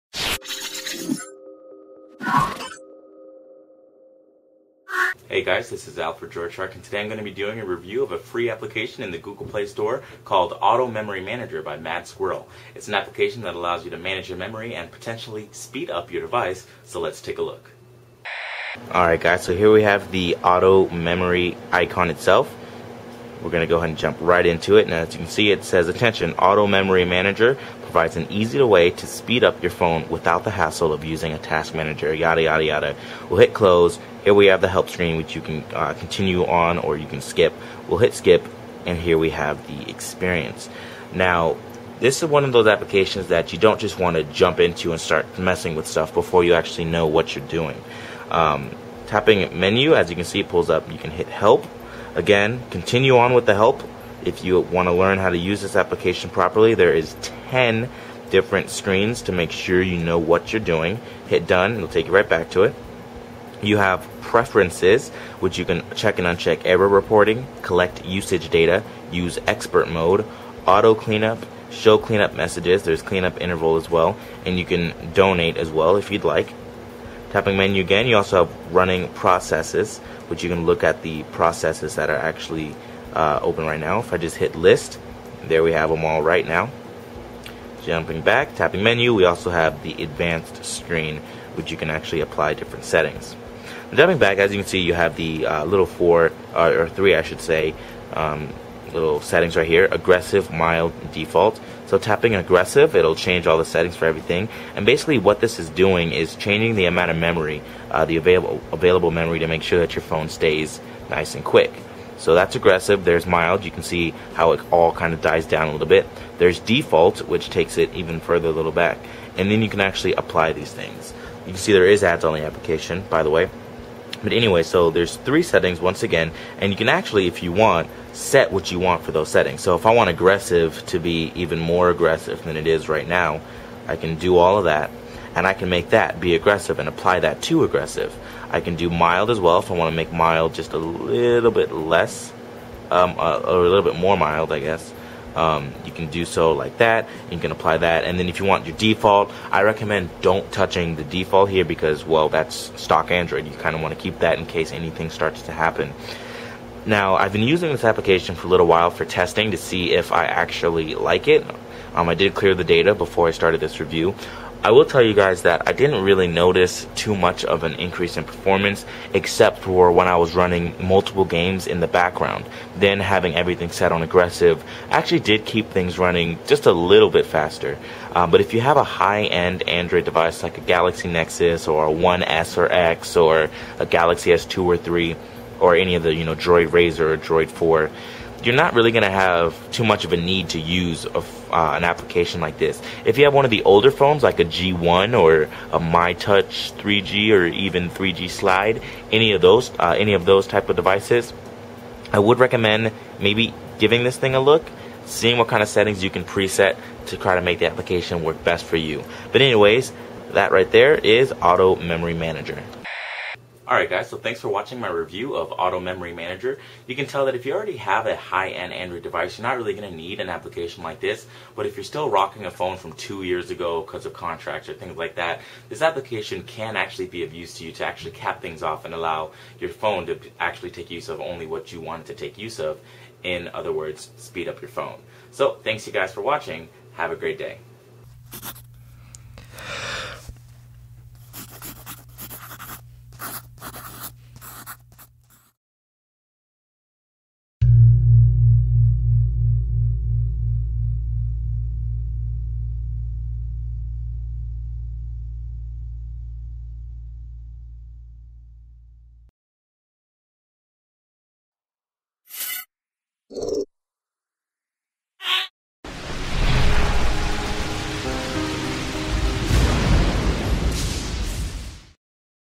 Auto Memory Manager by iolo technologies LLC. | Droidshark.com Video Review for Android
4/5 as reviewed by PCMag and included in PCMag's Top 100 Android Apps of 2013
Forget about "kill all" buttons and all the hassle. AMM lets you customize the out-of-memory manager settings on your phone so you don't have to do it yourself with task killers. This works on both rooted and unrooted phones. So just start it up and forget about it.
If you have any questions or find a bug/problem please email the developer. We will try to reply ASAP.
Donating or buying the Premium version will remove ads.
task killer minfree lowmemkiller AMM